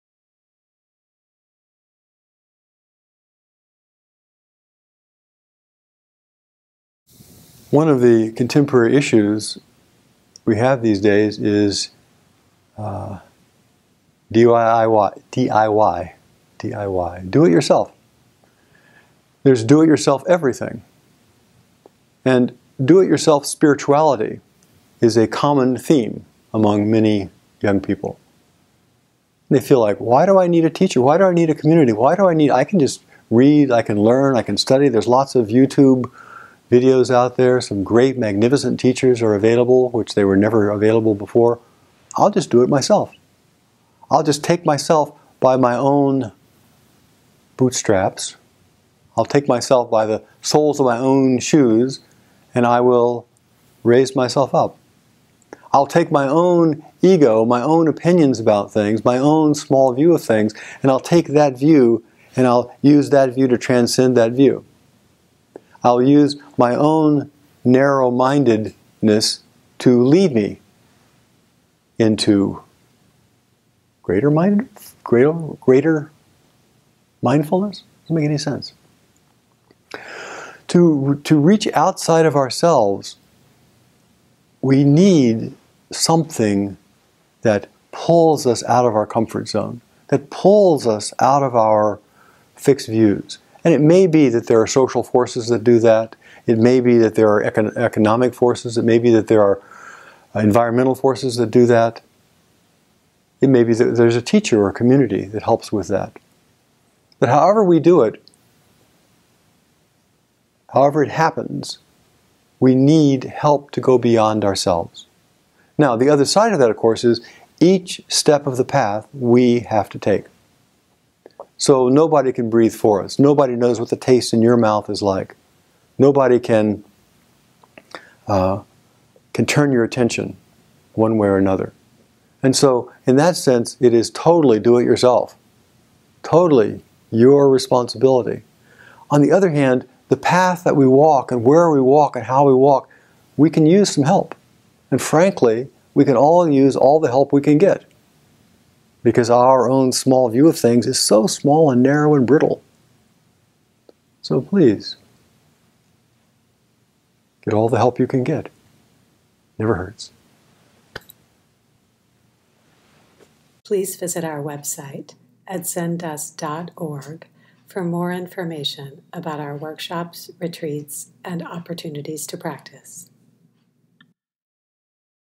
Don't "D-I-Y" the Dharma- Hogen Bays Roshi
Hogen cautions us against diving into practice only under the guidance of our self-created regimens.  Many supports exist for practice such as books, videos, and dharma talks, and these can help sustain us.  However, we cannot create our own spiritual practice by cobbling these together.  The self is not the only resource for transcending the self.